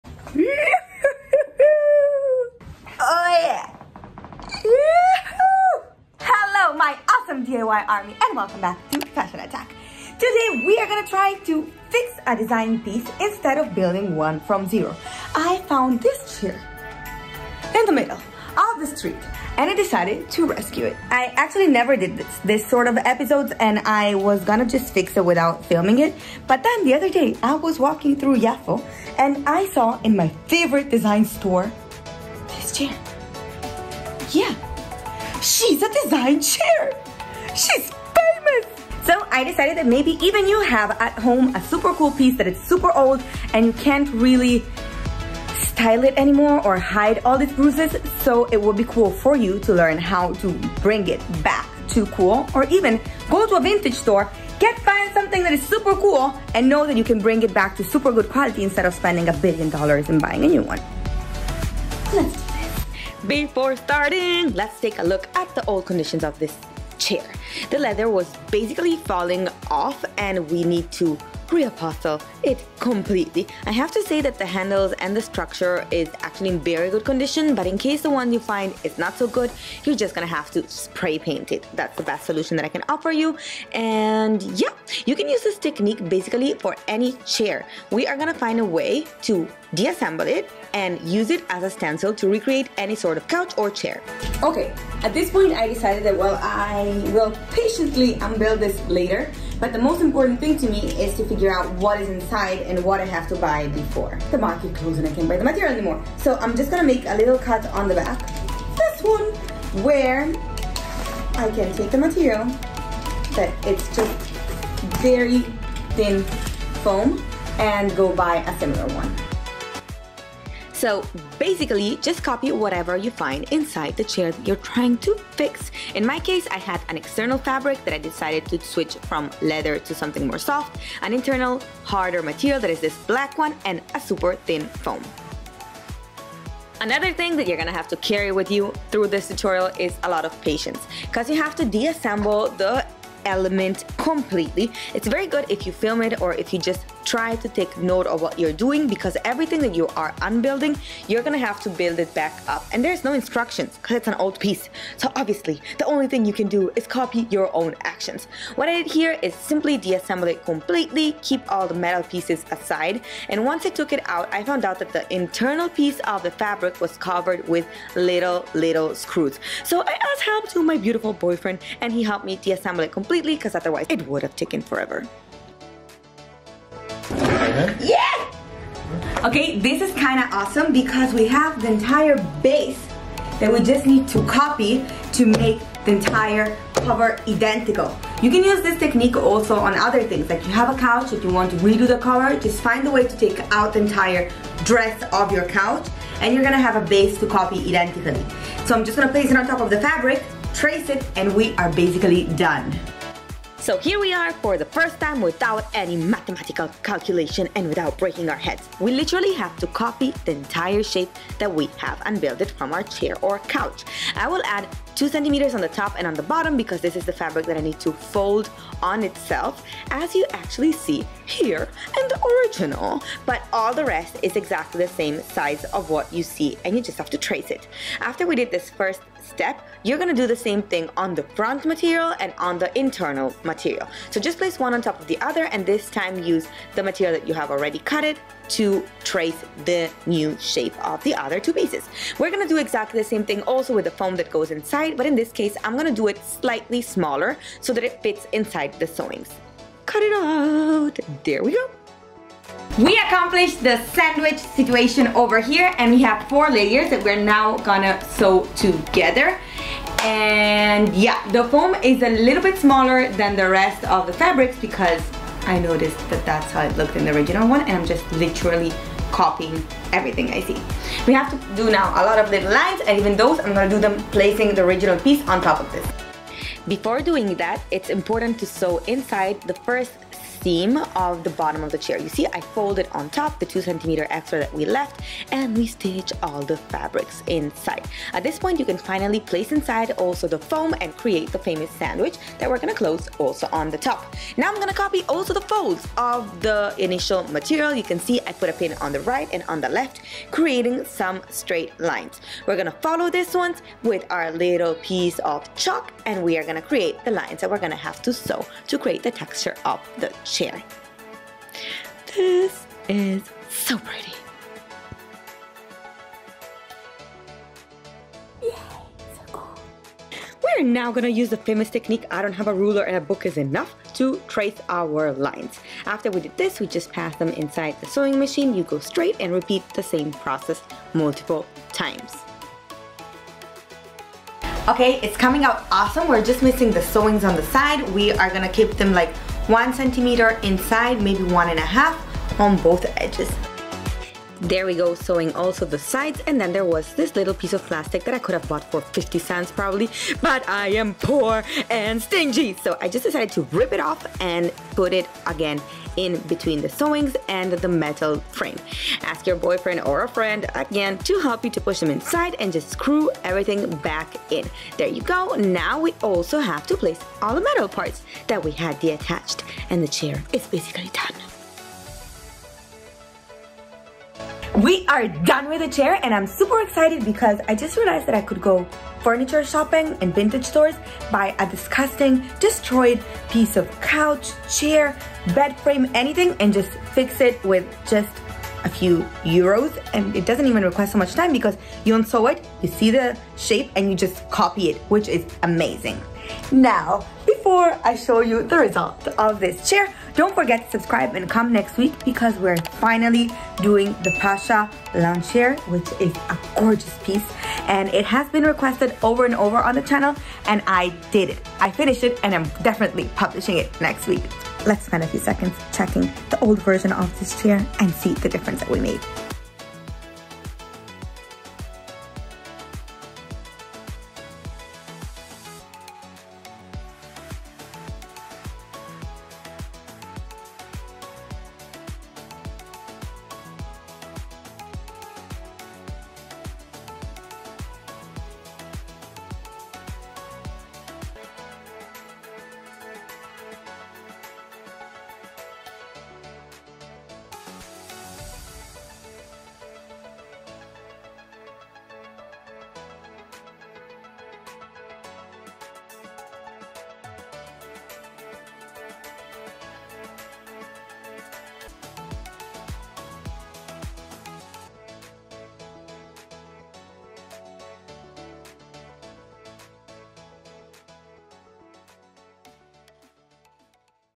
DIY: HOW TO REUPHOLSTER A CHAIR / office chair / furniture flipping
Hello DIY army
Today we are gonna see how to reupholster a chair a couch anything that was in bad conditions and you need to bring back to life.
This DIY is gonna show you an easy technique to fix old but amazing pieces of furniture.

I found this office chair abandoned on the street and i decided I CAN SAVE YOU GIRL!!!
heheh i'm so excided about this furniture flipping

what i used was:
- 2 Alcantara
- thin foam

LET´S BE FRIENDS!